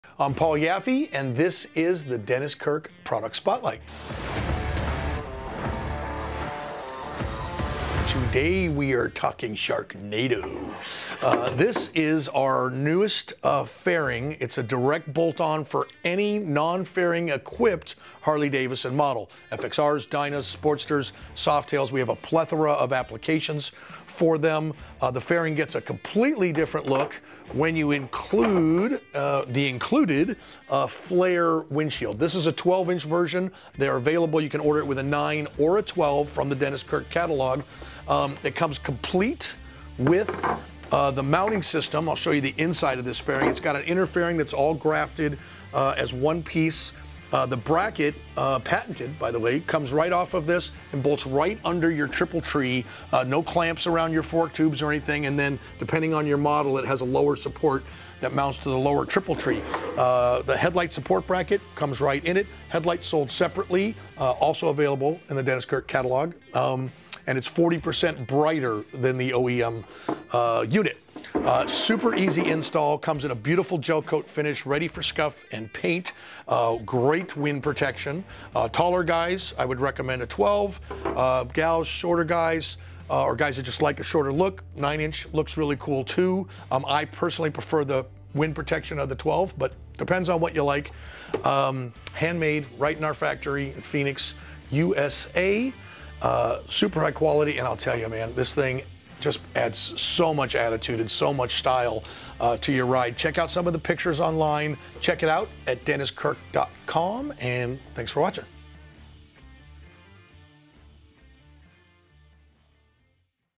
Sharknado Fairing from Paul Yaffe | DK Product Spotlight
Do you have fairing envy? Paul Yaffe and Bagger Nation have your back. They developed the Sharknado fairing kits to equip any non-fairing Harley with a mini Shark Nose style fairing. Follow along in this DK Product Spotlight to learn more about the Sharknado Fairing Kits.

Yaffe and company designed the Sharknado fairing kits to provide a unique and aggressive style to non-fairing equipped bikes. You also get the benefit of added wind protection when you install the Sharknado. The fairings include the patent-pending EZ Mount system, which makes installation a breeze and eliminates unsightly clamps and brackets.

The Sharknado is hand-built in Phoenix, Arizona by the talented professionals of Bagger Nation. The fairing is beautifully gel coated and ready for scuff-and-paint to match your bike. The fairing is compatible with the Klock Werks Flare windshields. It also utilizes the current Fat Bob style headlight for a great style.

Dennis Kirk has you covered with over 185,000 parts in-stock and ready to ship today!